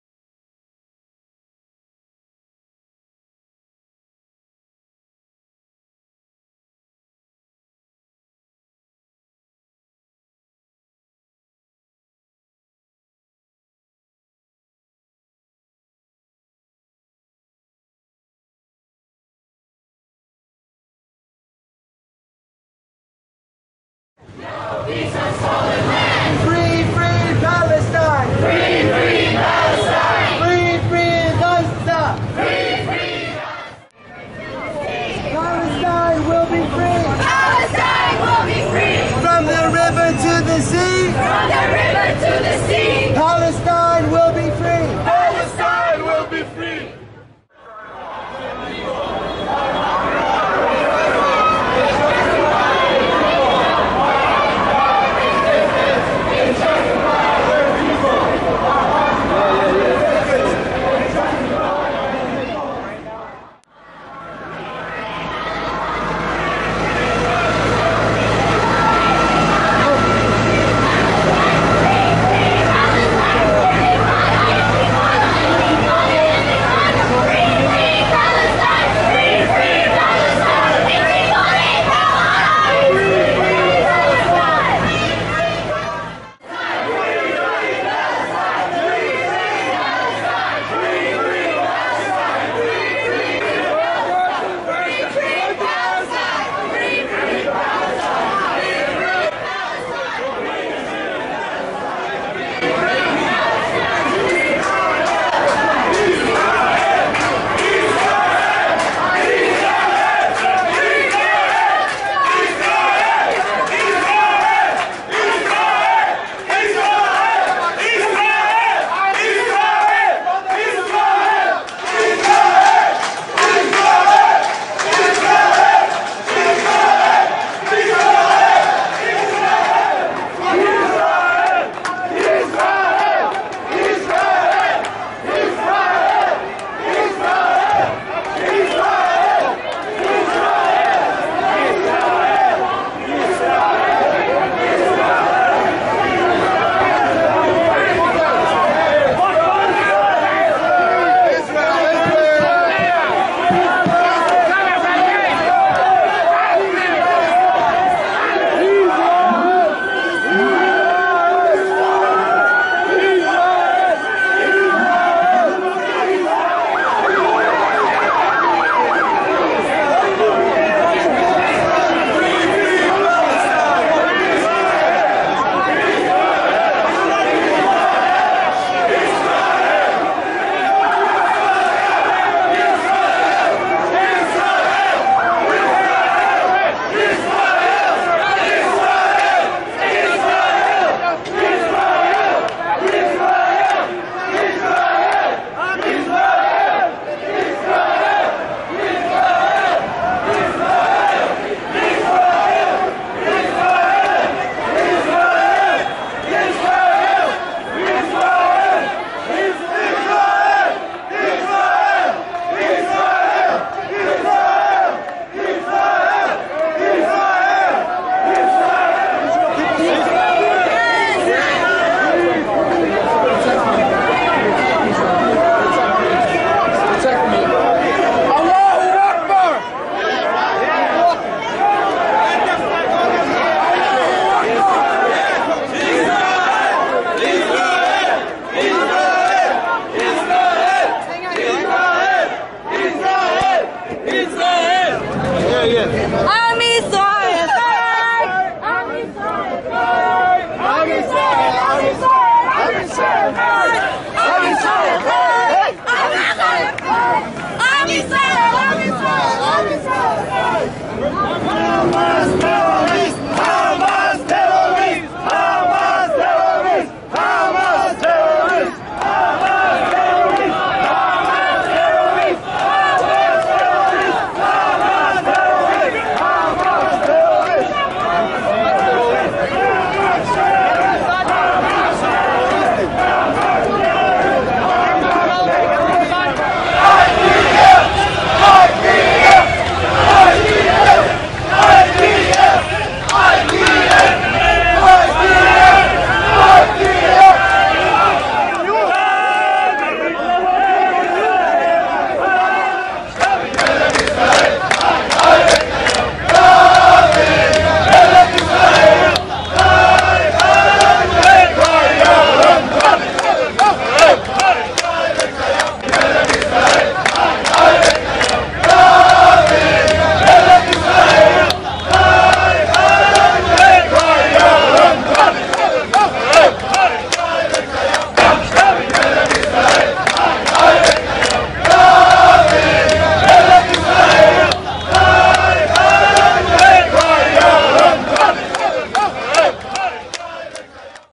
When anti-Israel protesters call for "intifada" on the streets of New York,
When anti-Israel protesters call for "intifada" on the streets of New York, thousands of Jews spontaneously start rally in support of the IDF and Israel. WATCH